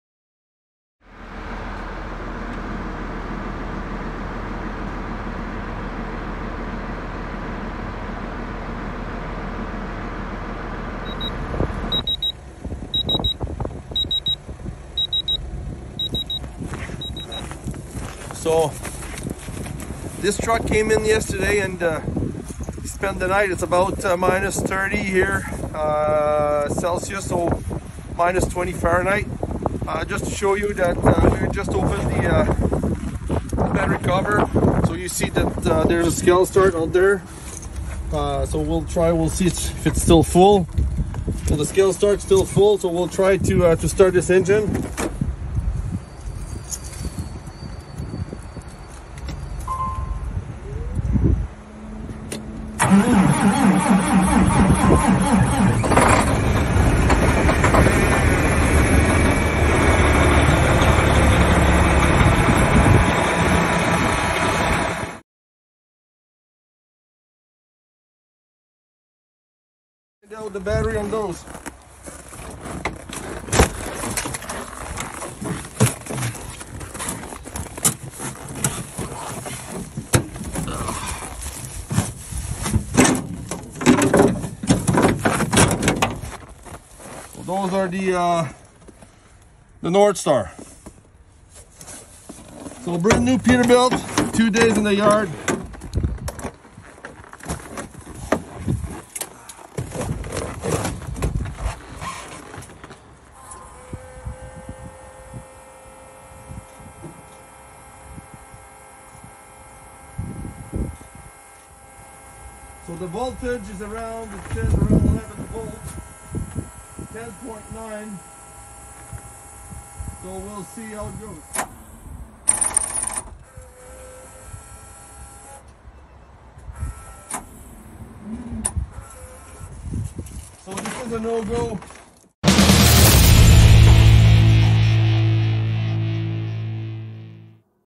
SkelStart ESM cranks an engine in freezing cold weather (-22°F / -33° C)
SkelStart delivers where lead acid batteries fail!

A special thanks to those who froze their butts with us making this test possible:
*Martin Lussier from Excellence Peterbilt for all his support and for the access to a new truck that early morning on a weekend.
*JF Favreau from UAP/NAPA HPVD, for being our cameraman and to see by himself the product performance vs lead acid battery.
*Ben Groulx from Robert Transport, who is always a step ahead of others in this industry.